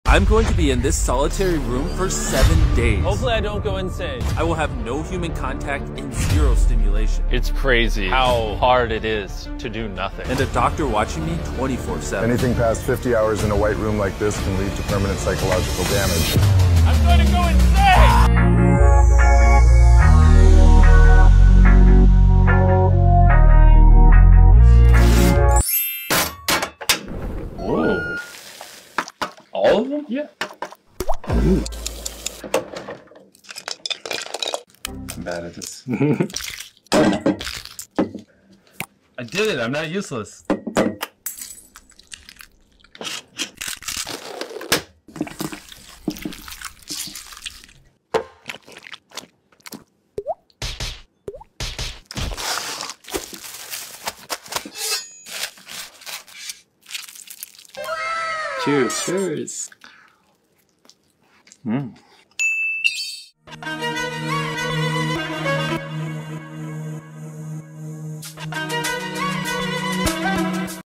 Days in the Life of MrBeast in Solitary Confinement
The days in the life of one of the most talked about personalities on the internet MrBeast